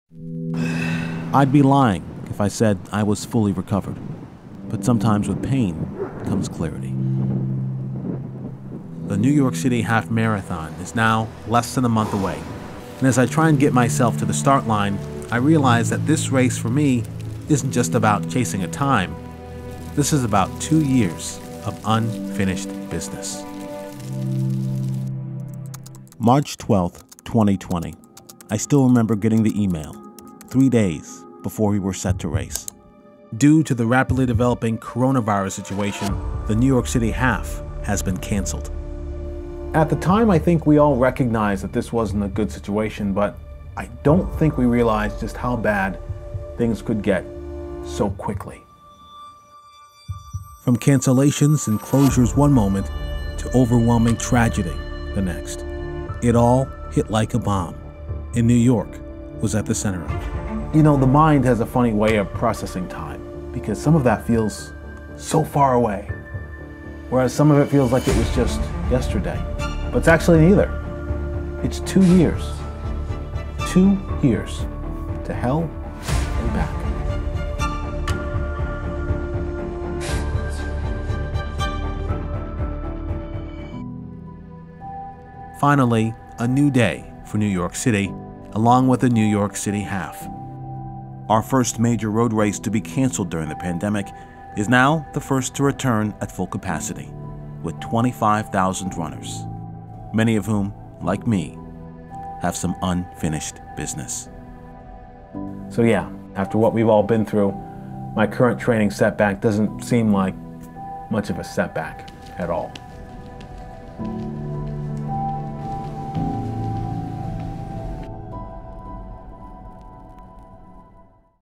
Harder than ANY race you've ever run | Fear, Tragedy and the 2022 NYC Half
This is the story of what might be the most important road race in New York City's history. The 2022 United NYC Half will not only mark the beginning of a new chapter 25,000 runners but for the entire city.